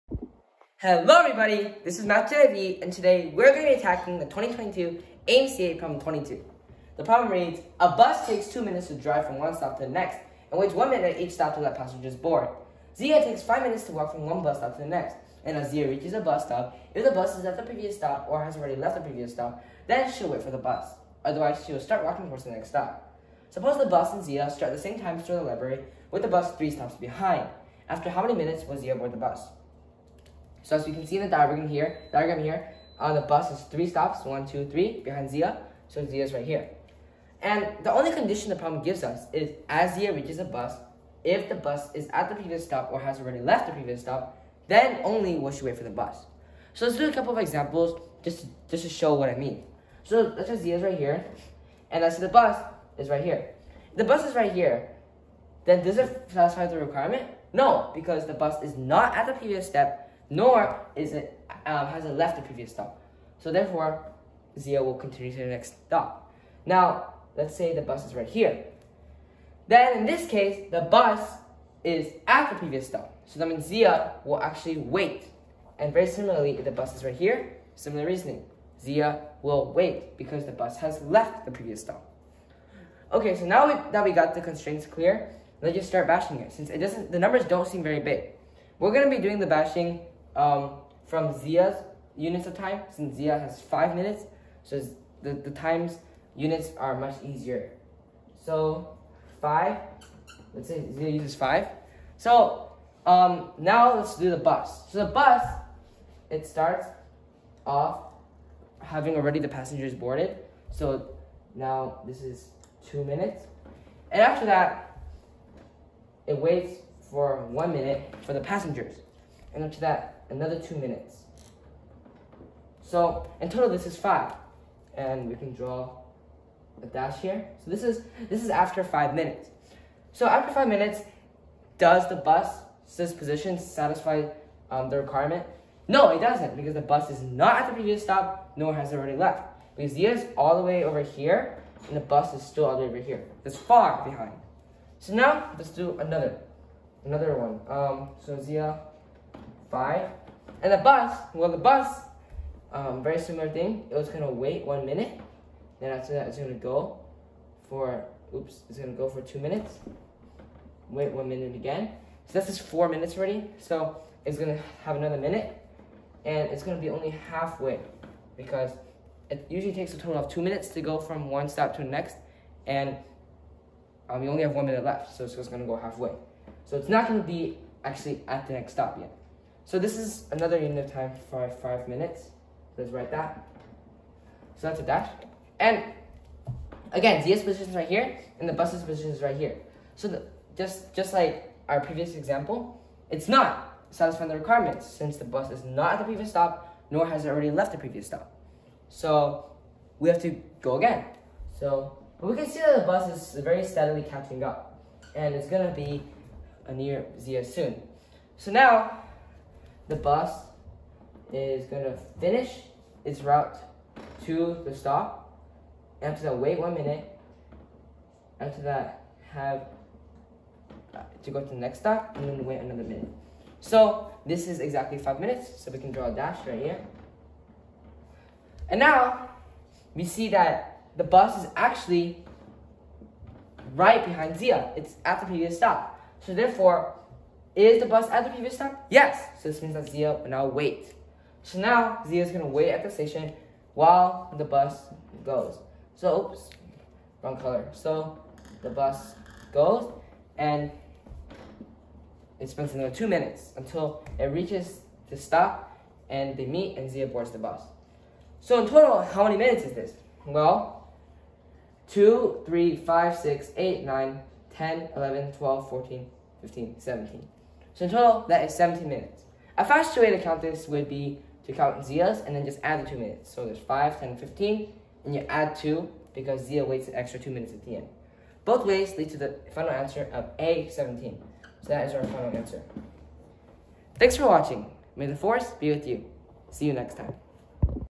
AMC8 2022 p22
Math Jedi V Vincent Lo will teach how to solve problem 22 on 2022 AMC8 competition